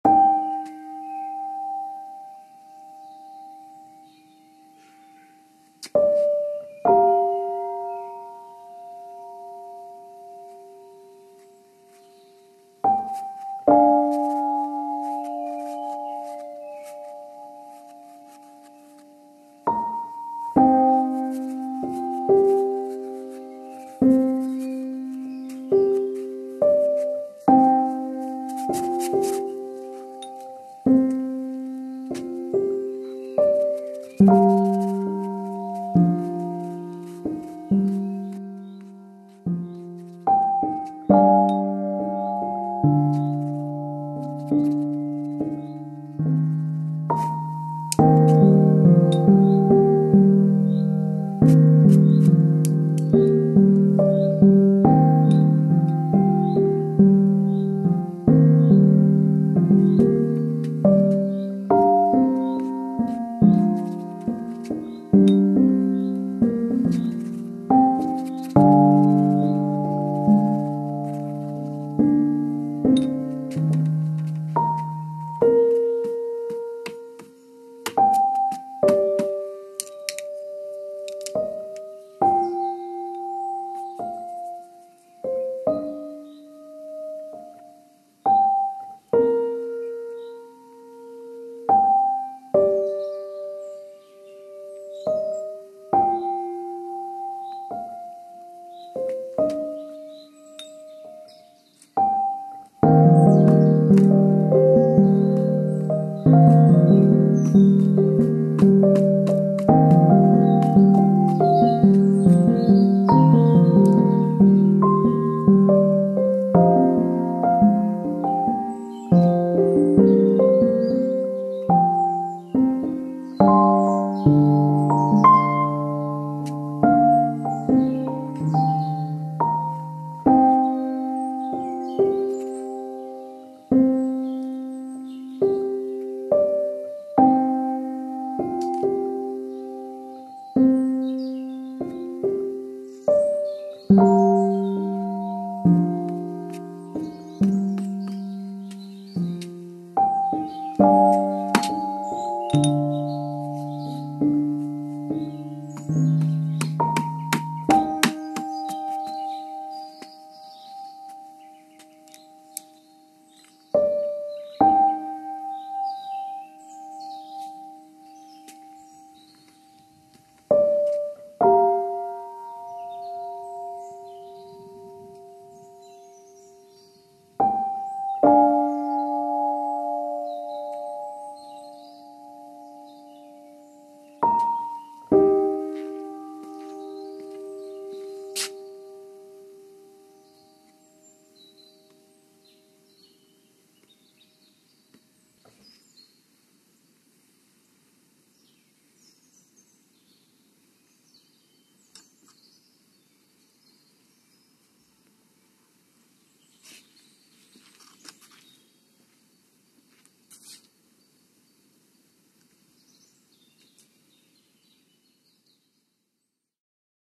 Painting the Almond Tree at Sunset
Painting the Almond tree in front of my studio in Provence.